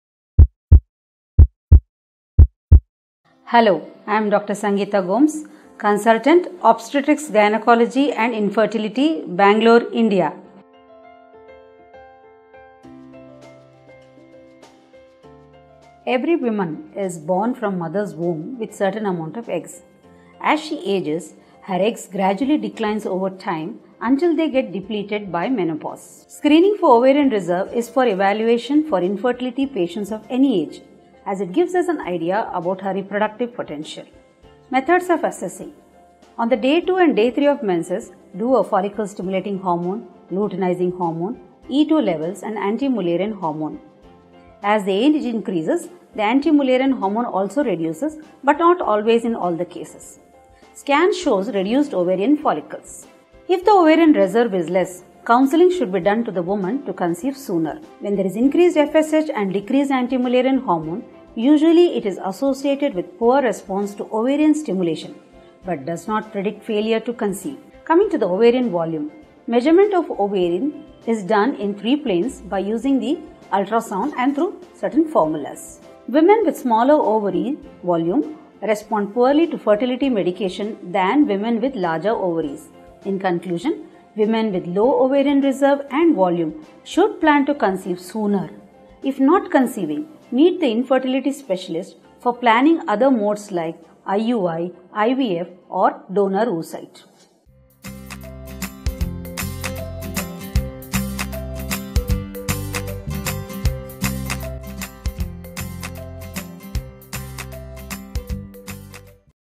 What is the normal ovarian reserve & volume? - Dr. Sangeeta Gomes
Every woman is born from mother’s womb with certain amount of eggs. As she ages her eggs gradually decreases by time until they get depleted by menopause. Screening for ovarian reserve for evaluation of infertility of people of any age and it gives us an idea about her reproductive potential. On the day 2 and day 3 of menses do a follicle stimulating hormone, leutenizing hormone, E2 levels and anti mullerian hormone. As the age increases the anti mullerian hormone also decreases but not in always all the cases. Scan shows reduced ovarian follicles. If the ovarian reserve is less counselling should be done to the woman to conceive sooner. When there is increased FSH and decreased anti mullerian hormone usually it is associated with poor response to ovarian stimulation but does not predict failure to conceive. Coming to the ovarian volume, measurement of the ovary is done in 3 planes by using the ultrasound and through certain formulas. Women with smaller ovary volume respond poorer to fertility medication than women with larger ovaries. Women with low ovarian reserve and volume should plan to conceive sooner. If not conceiving meet the infertility specialist for planning other modes like IUI, IVF or donor oocyte.